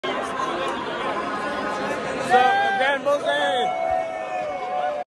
Dan Bilzerian At Jake Paul Anderson Silva Fight 😱😱
Dan Bilzerian At Jake Paul Anderson Silva Fight i got to see dan bilzerian at the jake paul fight jake paul won idk who dan bilzerian was going for in the fight the boxing match 2022

GEAR:
OUR CAMERA

OUR MIC

OUR LENS

AARON'S PHONE

Learn All Kinds Of How To's On This Channel If You Read All This,